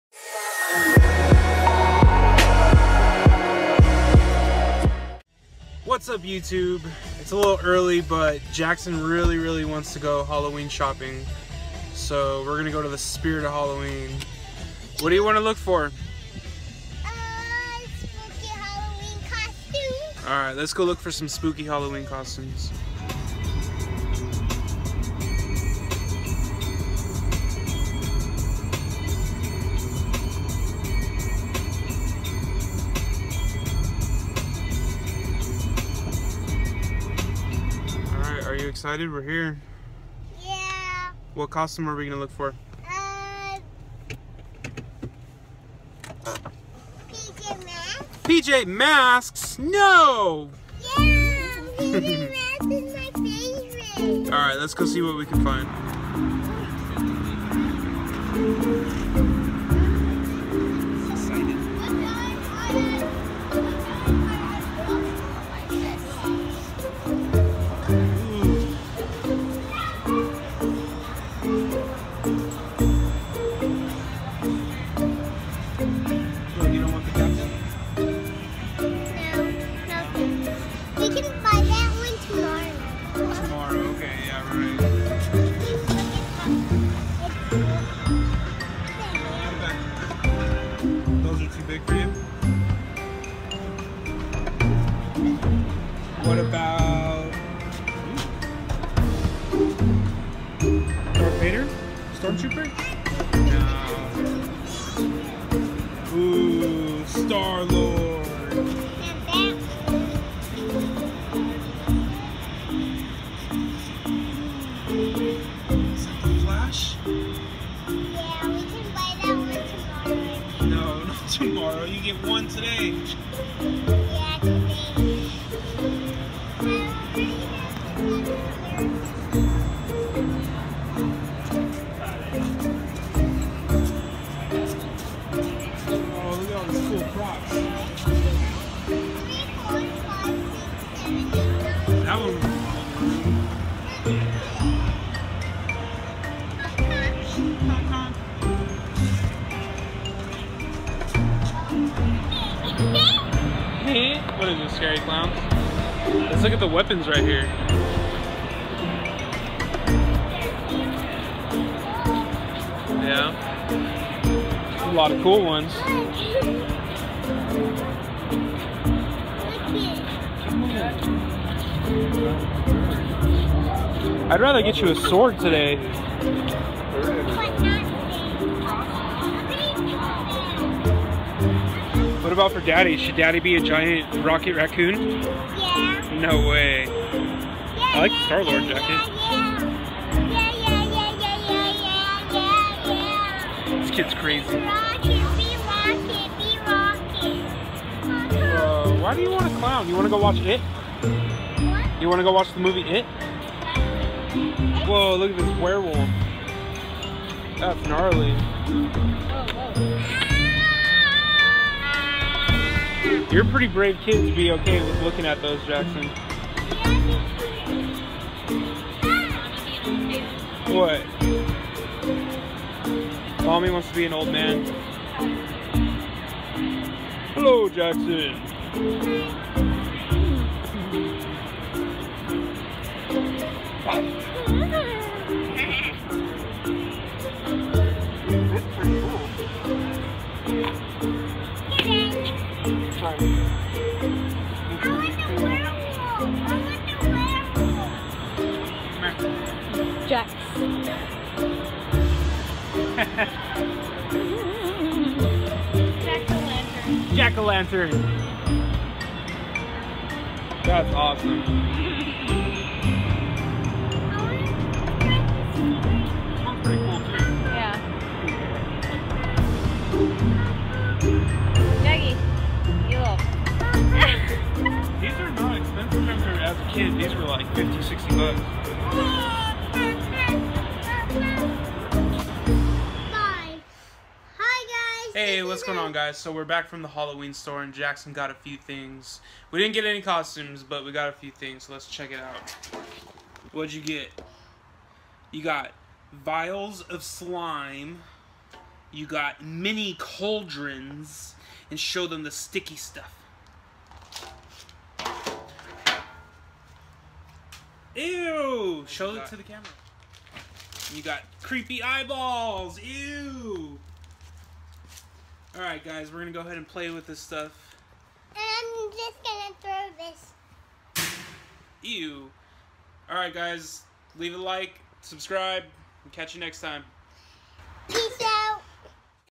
A quick look Spirit of Halloween 2017
taking Jaxson to the spirit of Halloween store to take a quick look at the latest costumes of 2017. join my channel for the latest reviews of action figures, movies, comics and more.